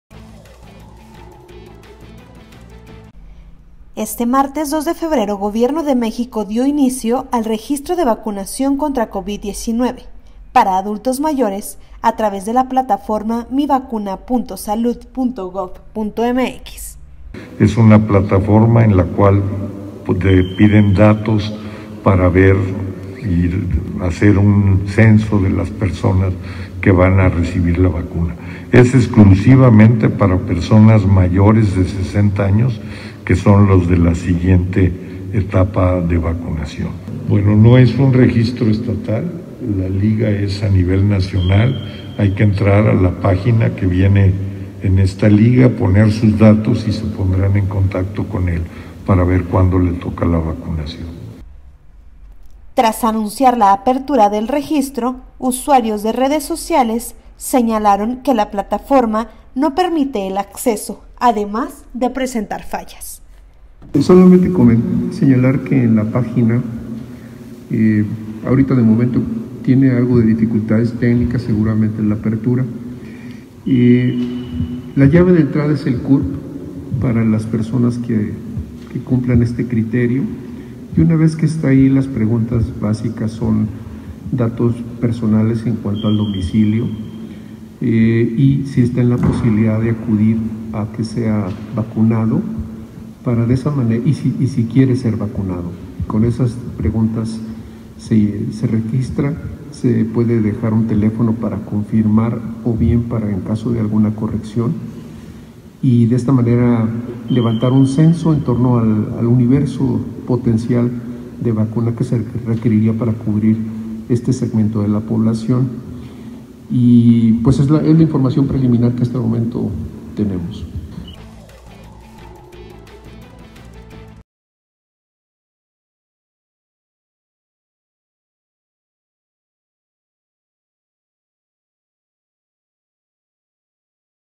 Con fallas en el sitio web, inició el registro de vacunación contra covid para adultos mayores.
Este martes dos de febrero, gobierno de México dio inició el registro de vacunación contra Covid-19 para adultos mayores a través de la plataforma mivacuna.salud.gob.mx. Tras anunciar la apertura del registro, usuarios de redes sociales señalaron que la plataforma no permite el acceso además de presentar fallas. Ante esto, la delegada de programas para el desarrollo en Zacatecas, Verónica Díaz Robles, informó que realizaran visitas domiciliarias para aquellas personas que no cuente con acceso a internet.